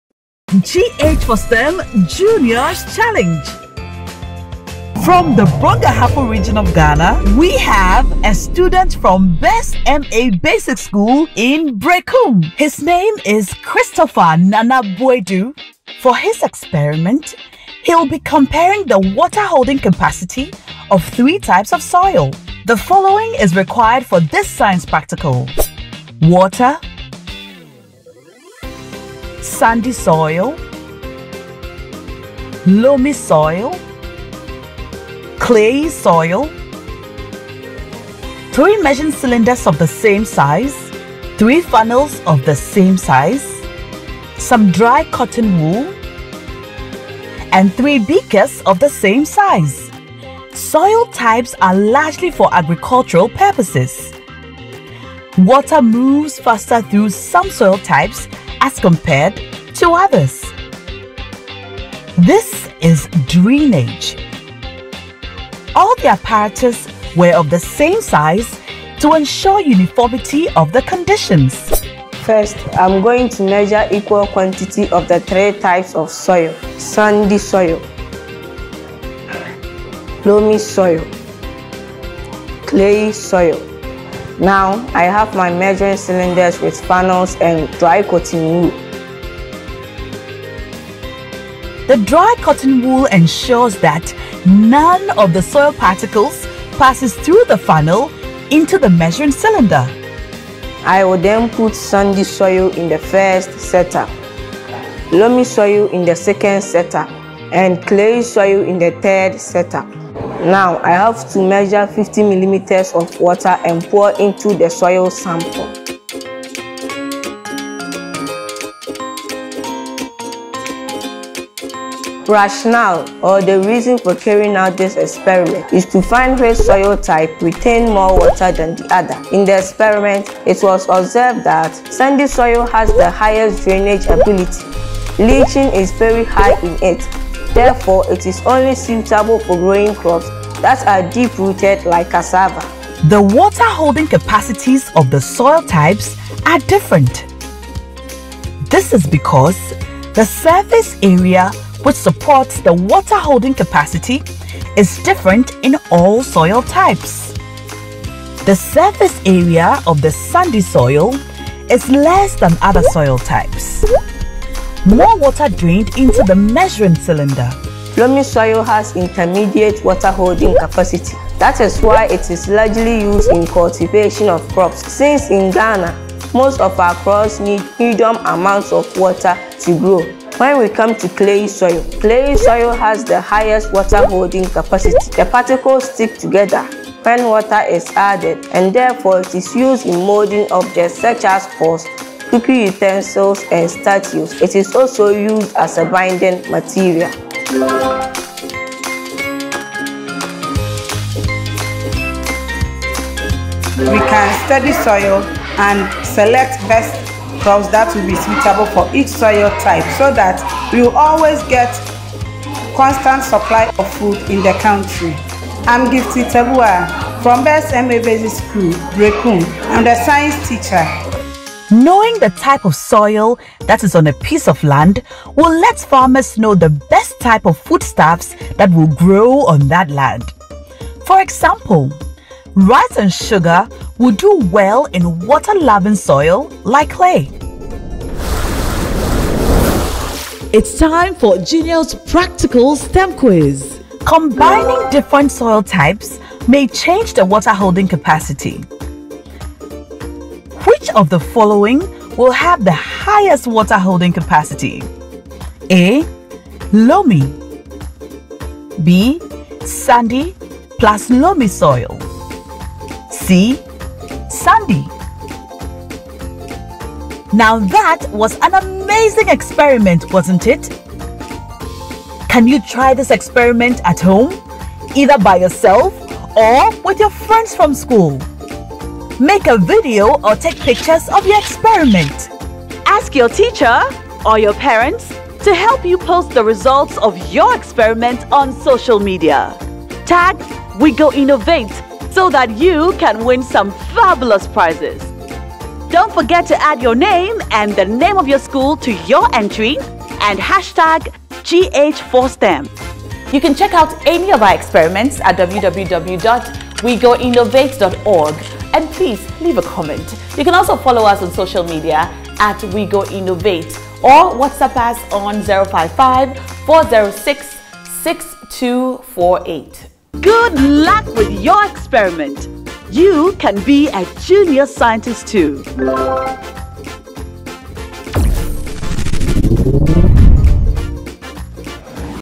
WATER HOLDING CAPACITY OF 3 TYPES OF SOIL - GH4STEM JUNEOS CHALLENGE
An investigation on the water holding capacity of 3 Types of Soil using water, sandy soil, loamy soil, clayey soil, dry cotton wool, 3 equal-sized measuring cylinders,  3 funnels of the same size, and 3 equal-sized beakers.

Student’s Name: CHRISTOPHER NANA BOADU
School: BESS MA BASIC SCHOOL